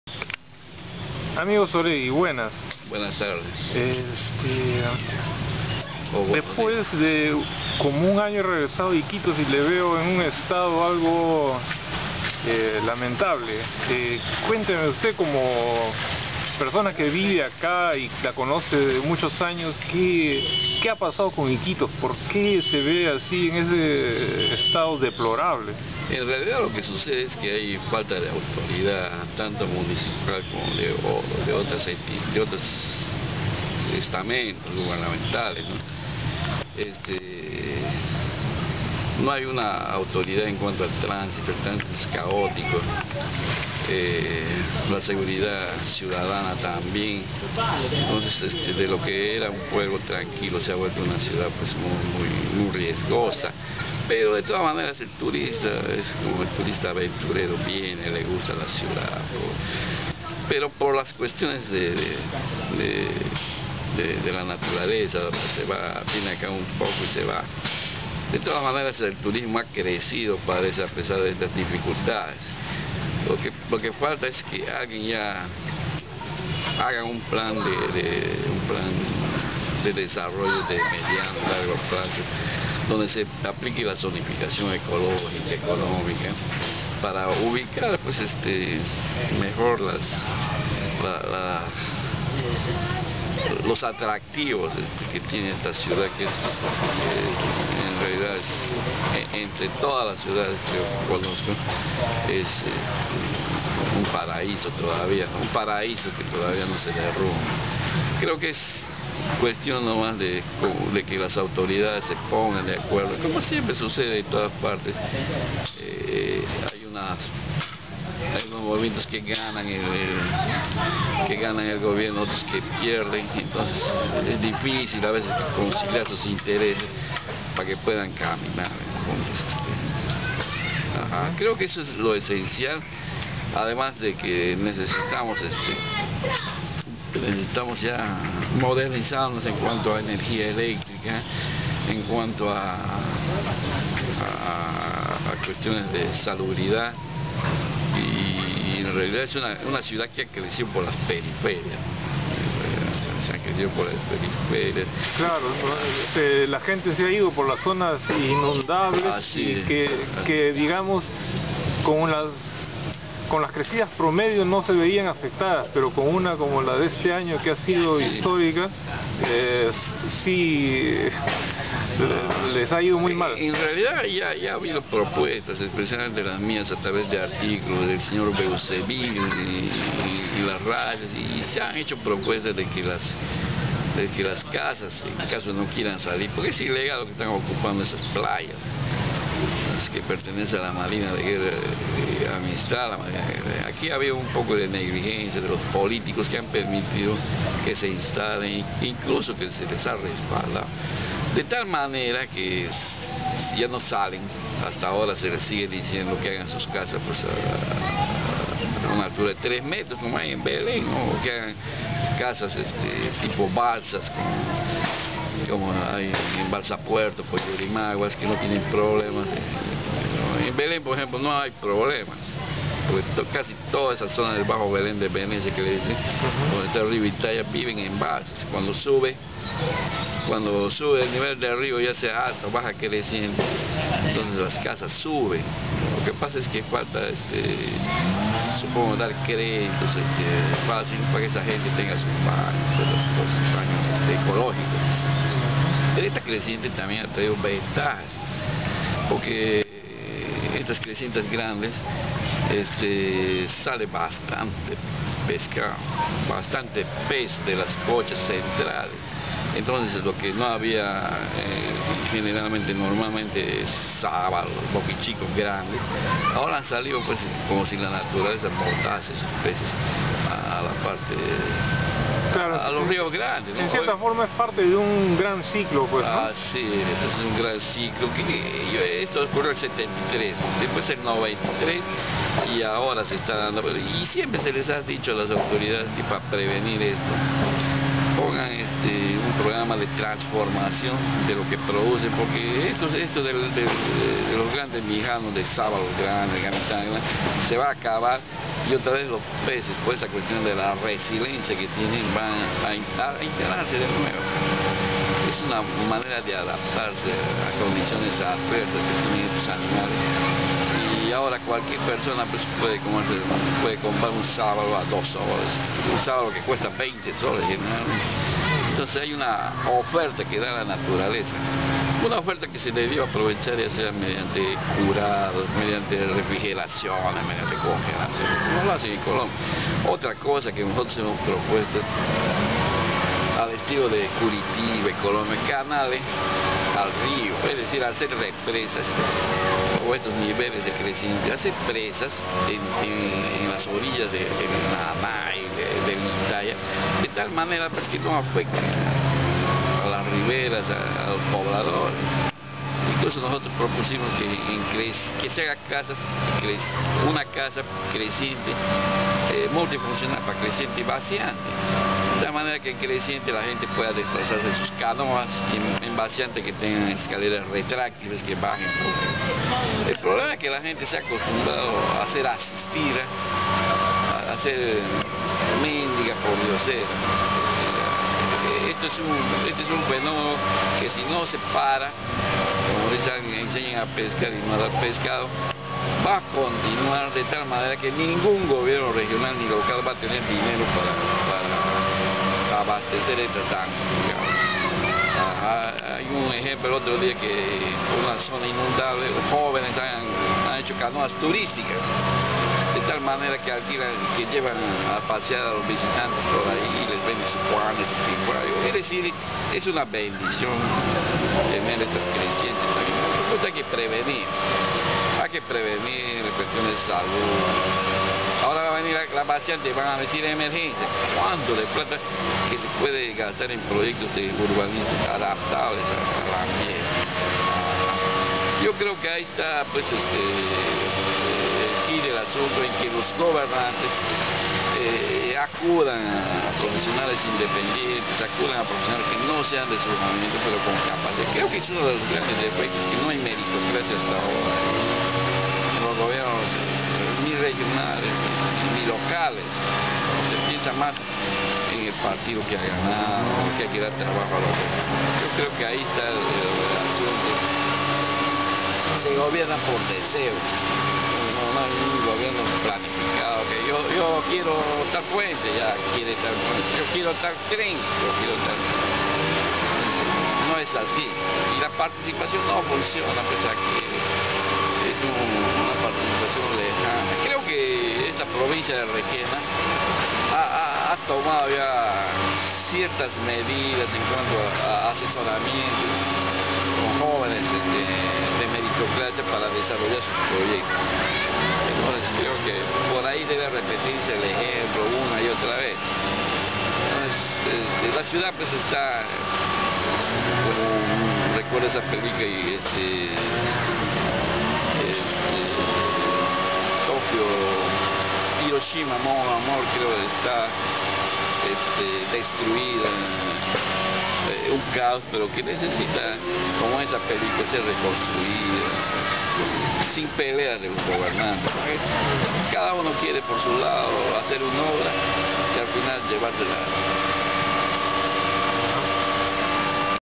Conversando con Juan Soregui, 1
Conversación con Juan Soregui, Biólogo, escritor. Parque Zonal, Iquitos, 3 de mayo del 2012, 12.21am.